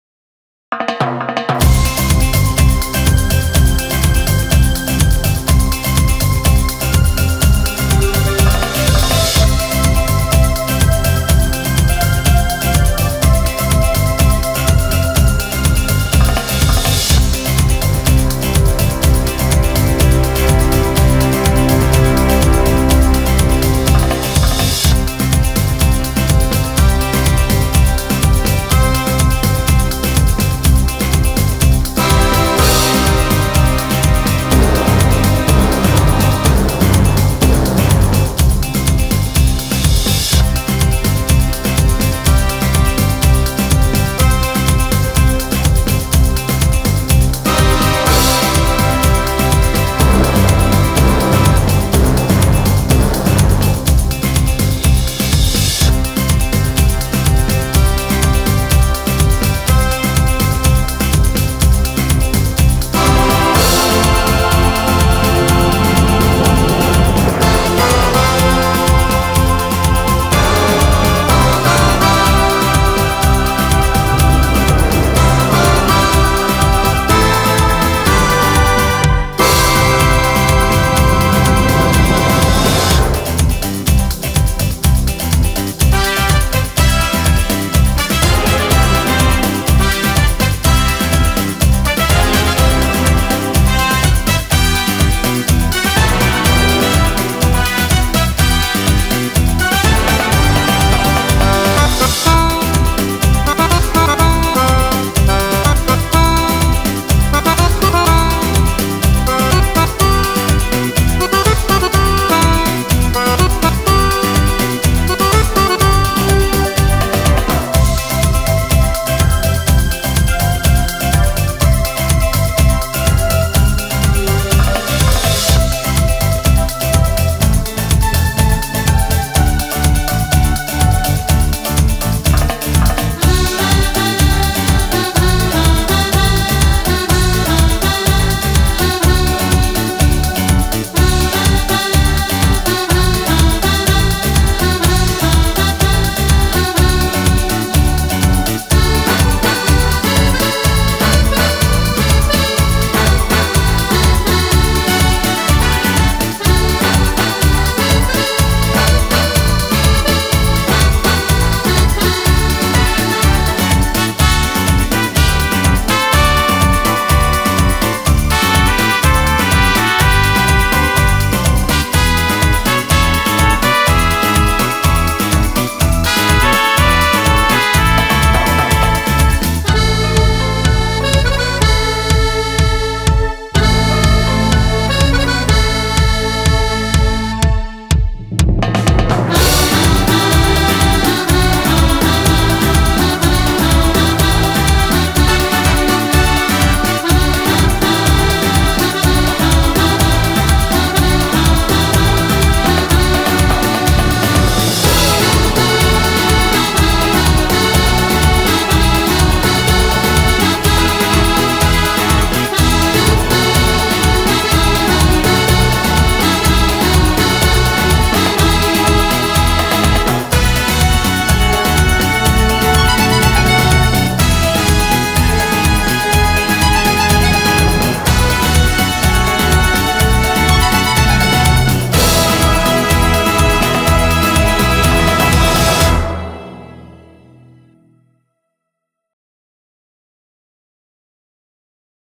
ZUMBA MEDLEY
Brani celebri riarrangiati a ritmo di zumba.